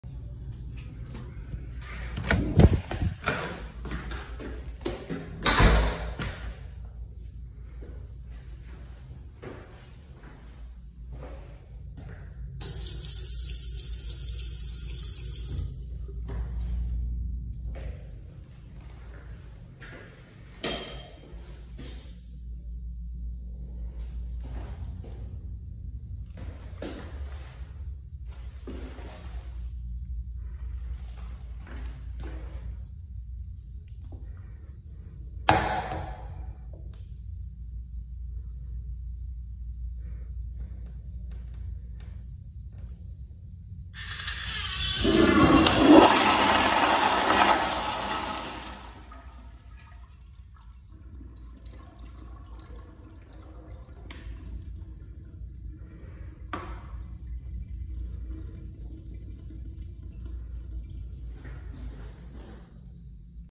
Target Unisex Restroom Reshoot
Same place as the last video. This restroom is also nice.

Eco Vantage: 10/10